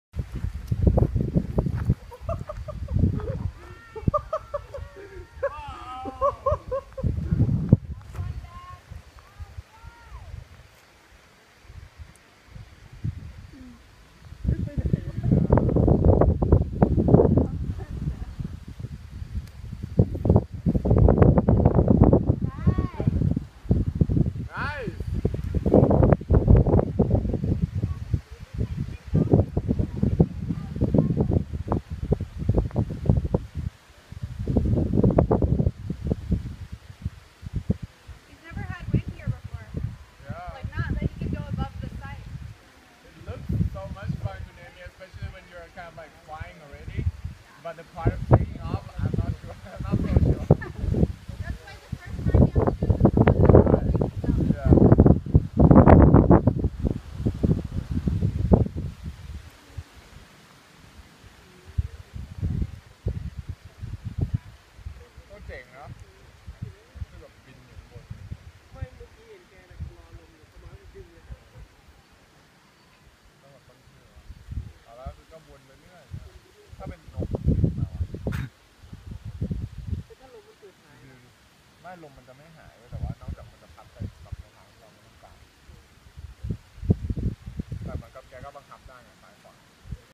Paragliding Doi Pha Phueng
June 2015 paragliding in Sangampheng Thailand.